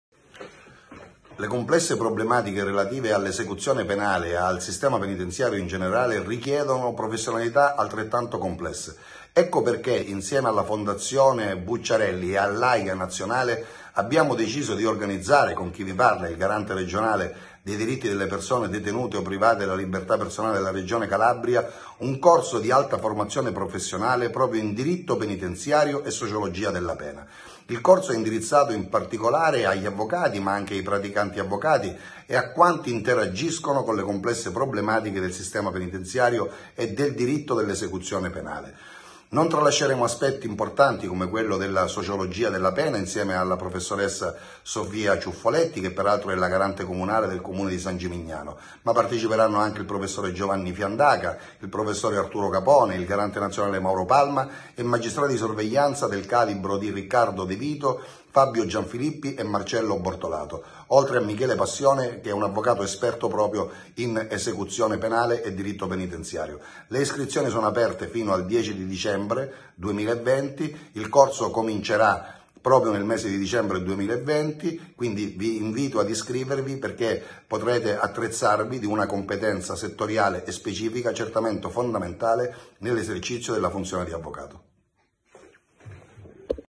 Presentazione del Corso in Diritto Penitenziario e Sociologia della pena - Avv. Agostino Siviglia
* PROGRAMMA DIDATTICO
Le lezioni si terranno il venerdì pomeriggio dalle ore 15:30 alle ore 17:30 ed il sabato mattina dalle ore 9:30 alle ore 11:30

Corso di Alta Formazione Professionale
In
“Diritto Penitenziario e Sociologia della Pena”

Modulo I
18 e 19 dicembre 2020
1) Il Diritto Penitenziario: evoluzione storica e riforme (Arturo Capone/Agostino Siviglia)
2) Le sanzioni disciplinari penitenziarie ed i rimedi giurisdizionali (Michele Passione)

Modulo II
15 e 16 gennaio 2021
• L’accesso alle misure alternative alla detenzione (Marcello Bortolato)
1) Ostacoli normativi alla concessione dei benefici penitenziari: l’art. 4 bis OP (Giovanni Fiandaca)

Modulo III
 29 e 30 gennaio 2021
1) Il regime del 41 bis OP (Fabio Gianfilippi)
2) La giursdizionalizzazione del procedimento di Sorveglianza (Riccardo De Vito)

Modulo IV
 12 e 13 febbraio 2021
1) L’esecuzione penale minorile (Francesca Stilla)
2) Introduzione alla sociologia della pena (Sofia Ciuffoletti)

Modulo V
26 e 27 febbraio 2021
1) La Sociologia della Pena: perché punire (Sofia Ciuffolletti)
2) La  tutela dei diritti delle persone detenute nell’ottica risocializzante (Mauro Palma)

*Eventuali variazioni di date saranno tempestivamente comunicate.